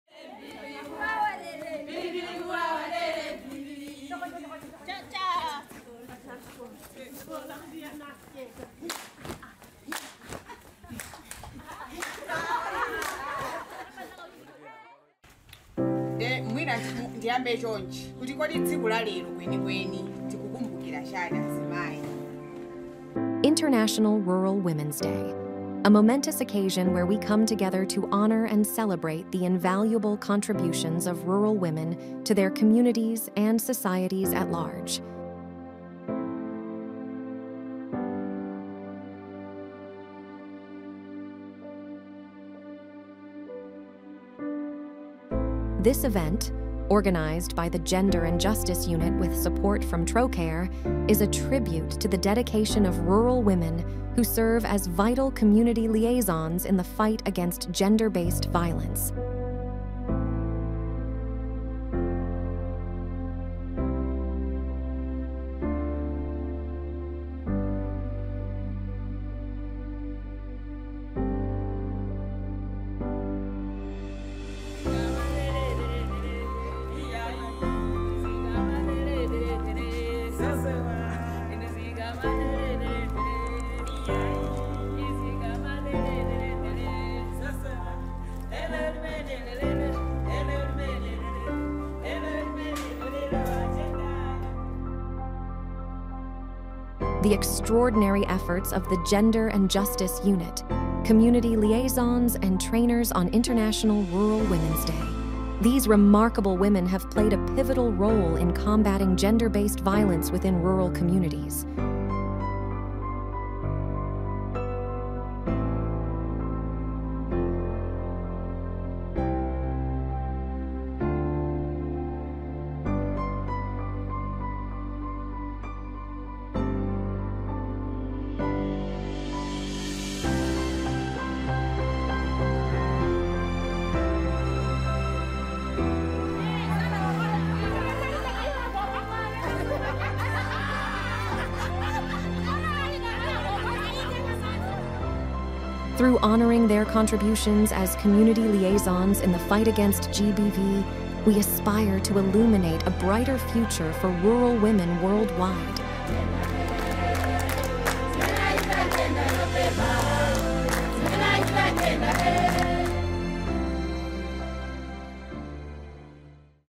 Highlights from the GJU Commemoration of International Day of Rural Women
🌟 Celebrating International Day of Rural Women & Honoring GBV Responders! 🌟

We had the privilege of commemorating International Day of Rural Women with an extraordinary event!

These incredible individuals serve as the backbone of our community, tirelessly working to combat gender-based violence and provide support to those in need. Their commitment to promoting gender equality and justice is truly inspiring, and today, we honoured their exceptional contributions.

The day was filled with fun and laughter as we recognized the vital role played by our GBV Responders. Team-building exercises brought us closer together, strengthening our bond as a community that stands against gender-based violence.

A highlight of the day was the award ceremony, where we acknowledged the four most active GBV liaisons. Their dedication and hard work have made a significant impact, and we are immensely proud of their achievements. 🏆🌟

This celebration would not have been possible without the generous support of Trocaire. We extend our heartfelt gratitude to Trocaire for their commitment to advancing gender equality and empowering rural women. Let's continue to stand together, support one another, and work towards a world free from gender-based violence. We are inspired by the resilience of rural women and the dedication of our GBV Responders, and together, we can create a brighter, more just future for all. 💪💖